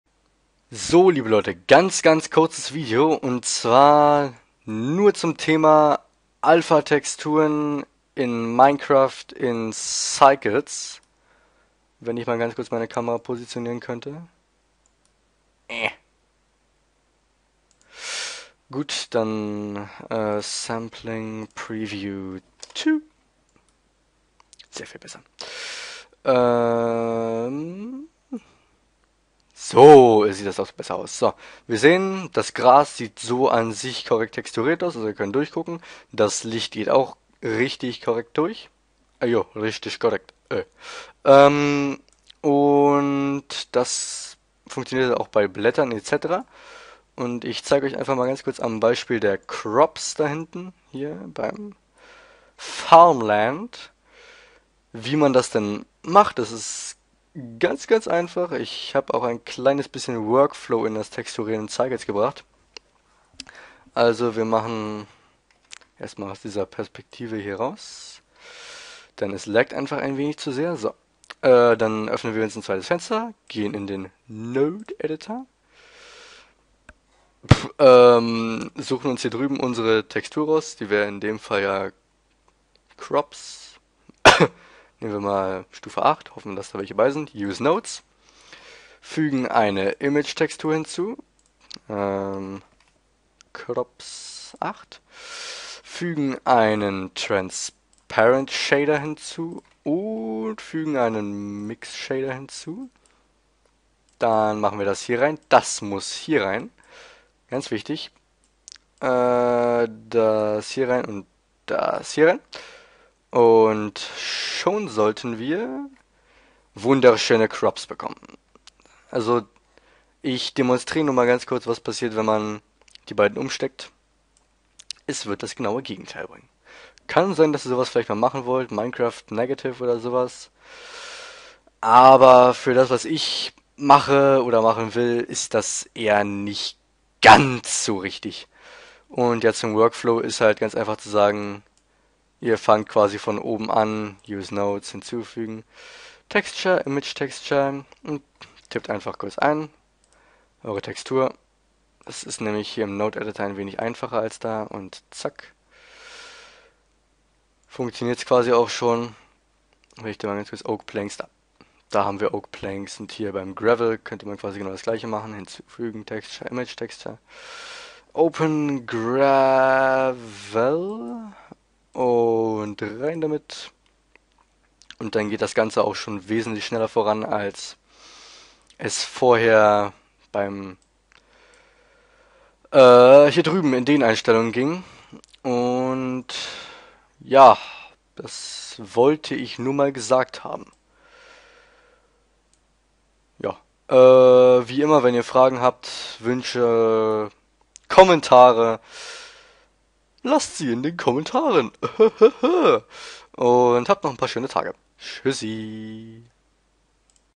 Minecraft meets Blender | Part 3.5 | jMc2Obj : Alphatexturen in Cylces + Workflow
Teil 3 der Serie "Minecraft meets Blender"
- Teil 3.5 : You are here

Geile Sache ... die nächste Animation wird 100%ig in Cycles gemacht werden :D